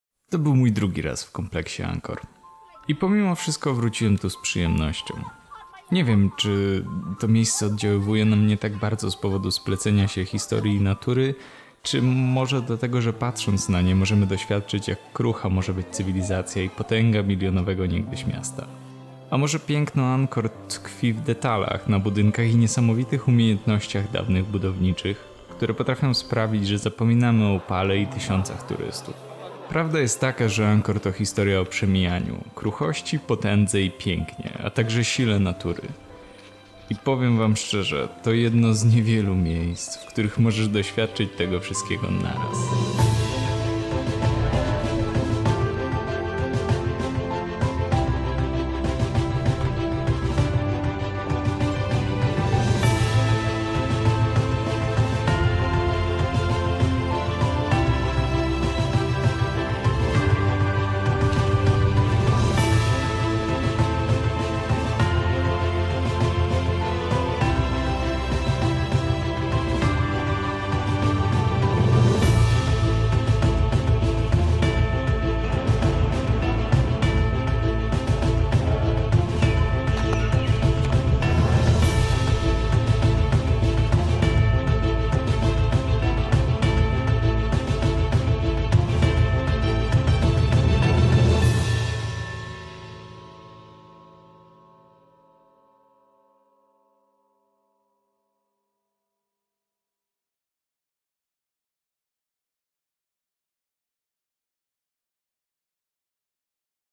Postcard: Angkor
Dlaczego warto pojechać do Angkor i zobaczyć je chociaż raz w życiu.
Oraz dlaczego ja tam wróciłem.